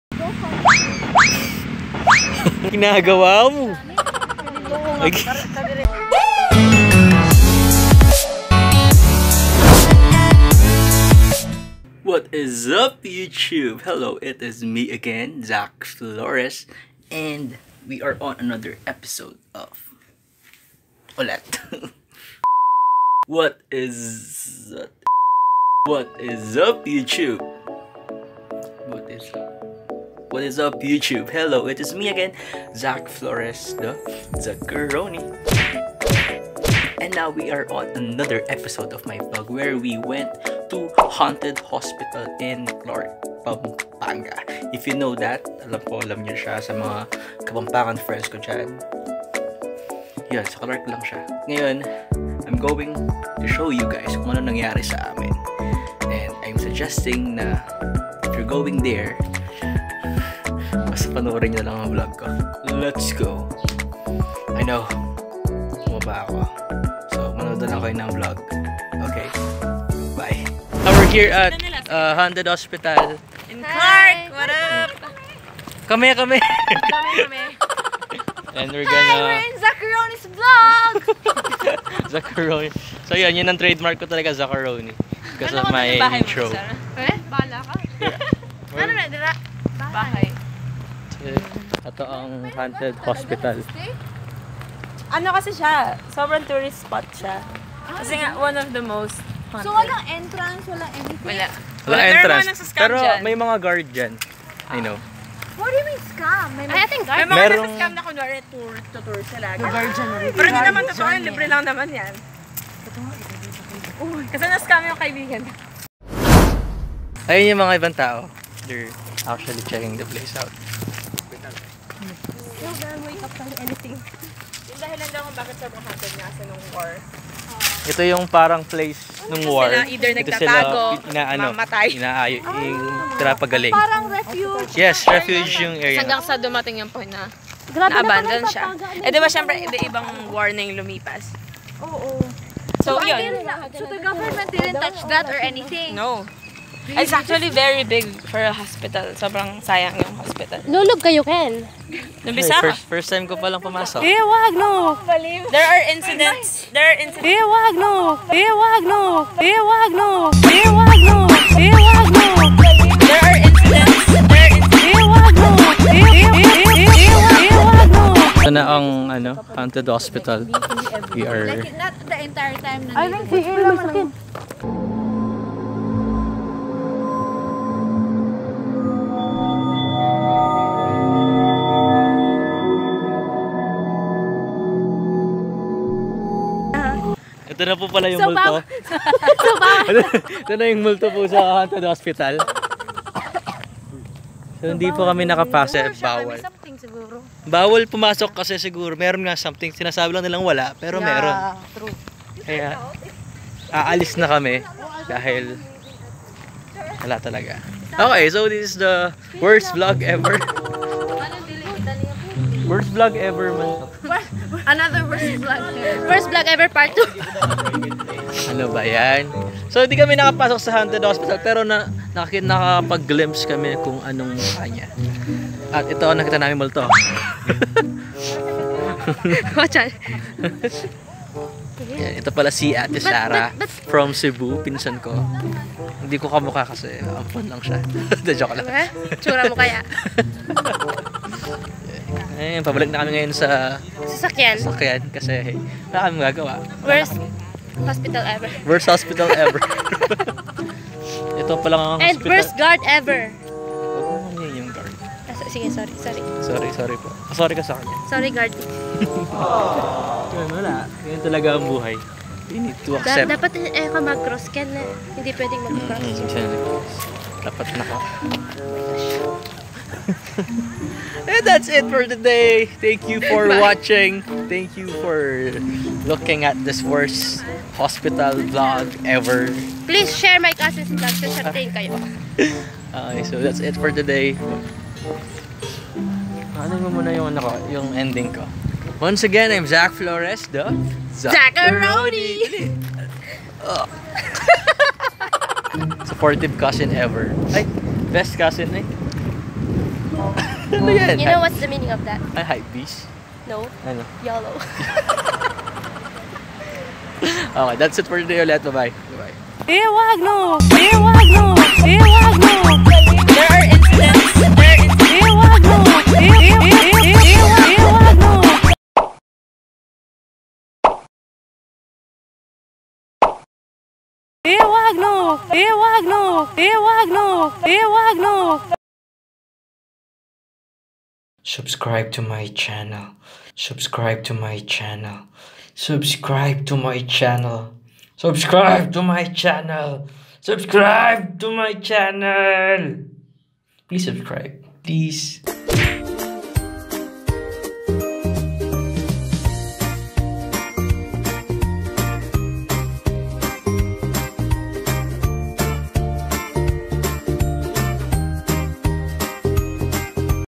SOMETHING WENT WRONG AT THE ABANDONED HOSPITAL // Zac Flores TV
this is just a glimpse of abandoned haunted hospital. well, go check out how it is managed this 2018.

The remains of an abandoned air force hospital located on a redeveloped Air Force base. ... Clark Air Base Hospital was used during WW2 and the Vietnam War. It now lays abandoned inside the Clark Freeport Zone.